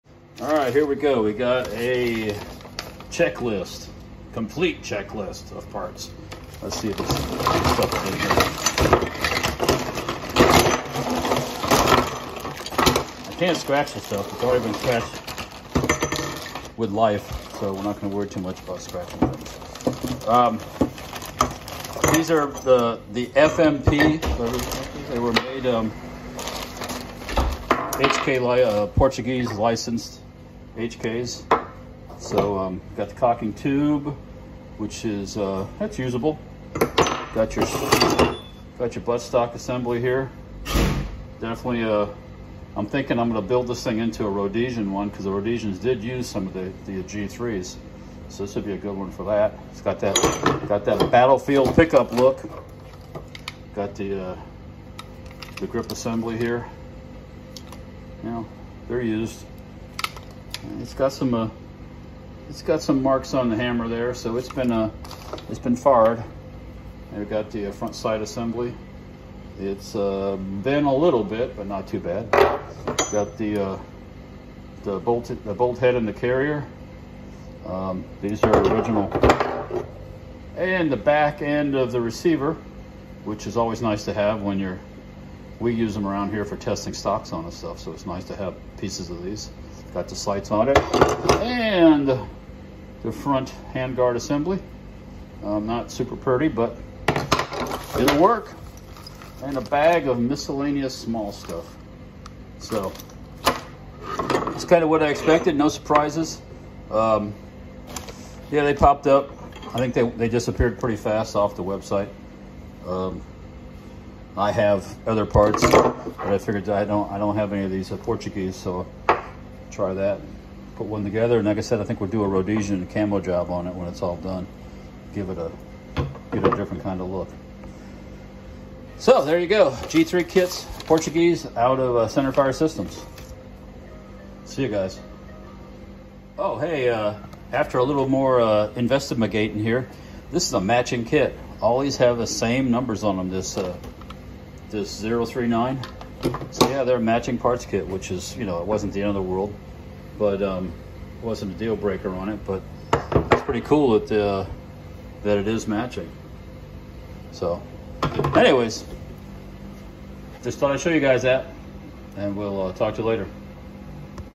G3 FMP Parts Kit
Centerfire Systems offered these G3 FMP kits recently …had to get one. All matching numbers which was a surprise.